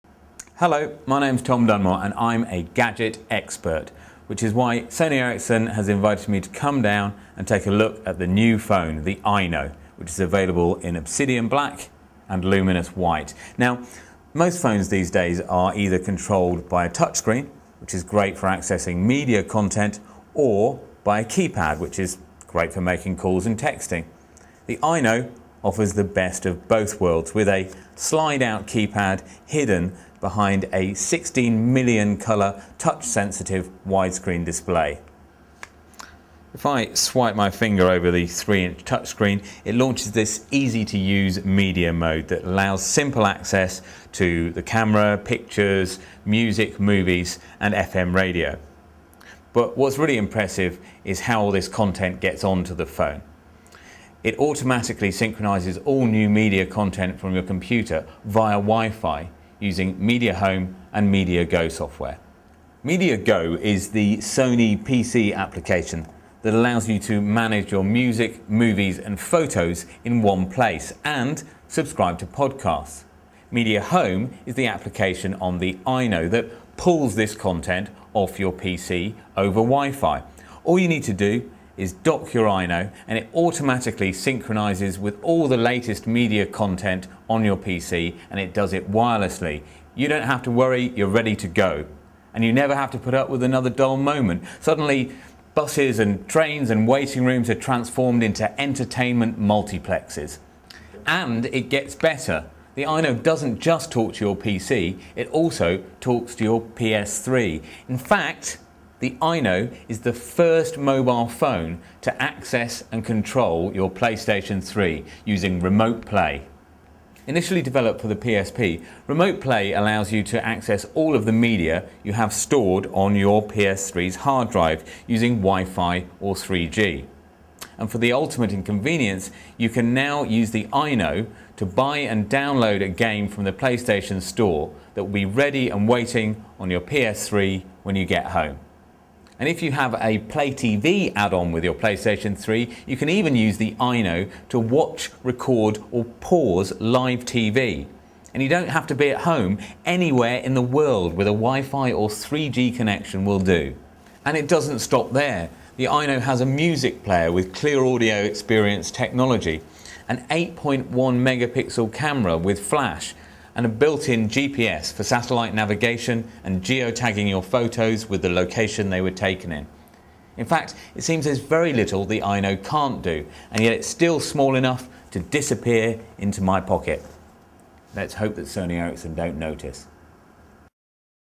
Tom Dunmore demonstrates the new Sony Ericsson Aino
Watch a demo of the Aino with Tom Dunmore, Consulting Editor at Stuff.